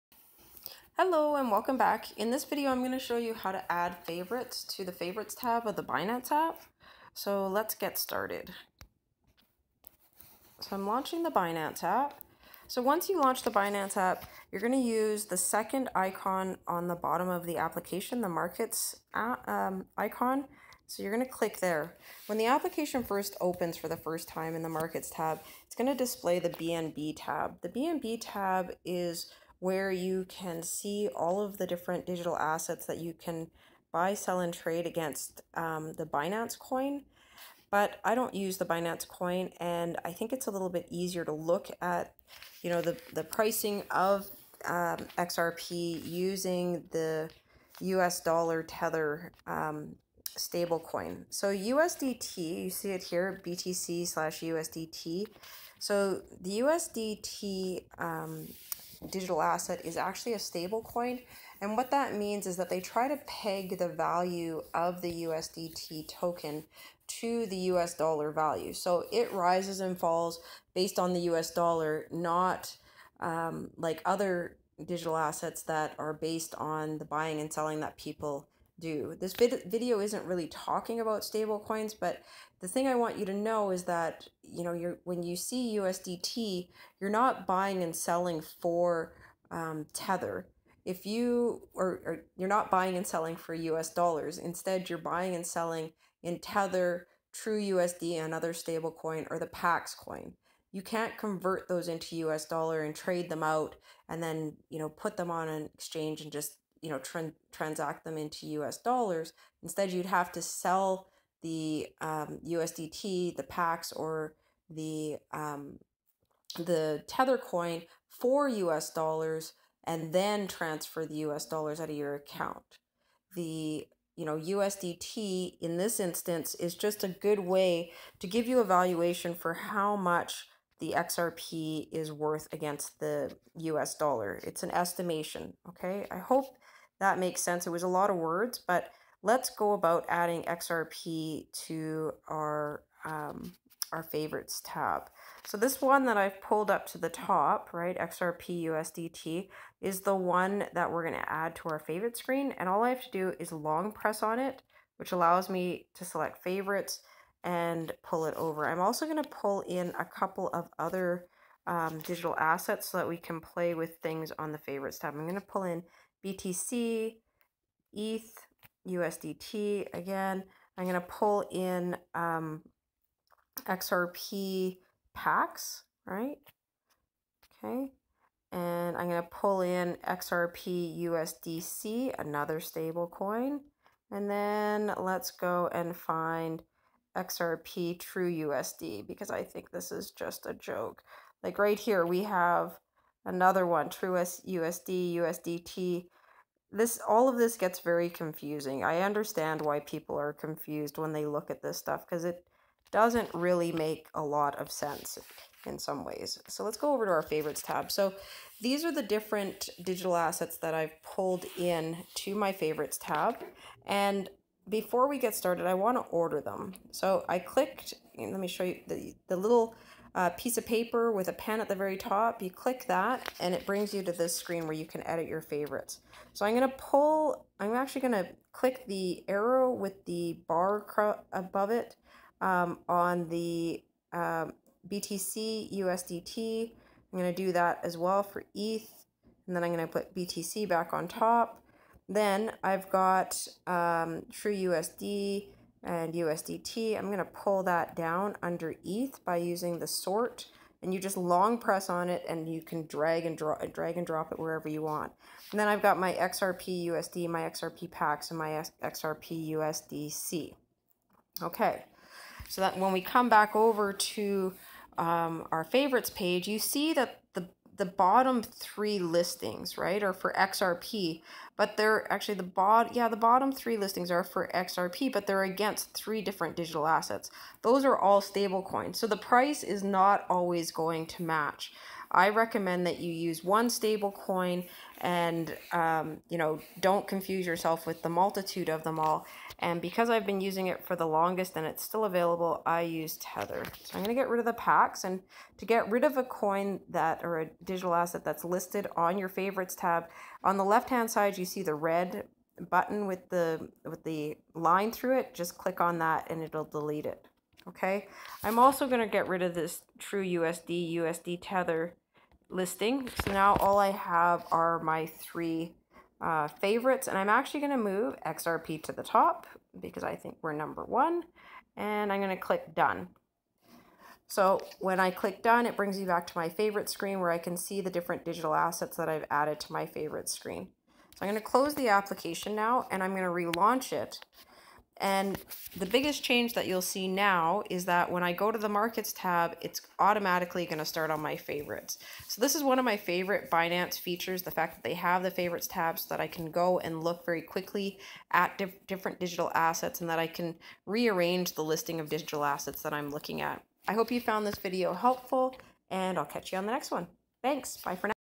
How to Configure Favorites in the Binance App
This video was created to show you how to configure favorites in the Binance app.

This video shows you how to do 3 things:
- how add digital assets to your Favorites area in the Binance app,
- how to arrange digital assets on the Binance Favorites tab,
- how to delete digital assets from the Binance Favorites listing.

The ability to organize a group of digital assets in one place is one of my favorite features of the binance application.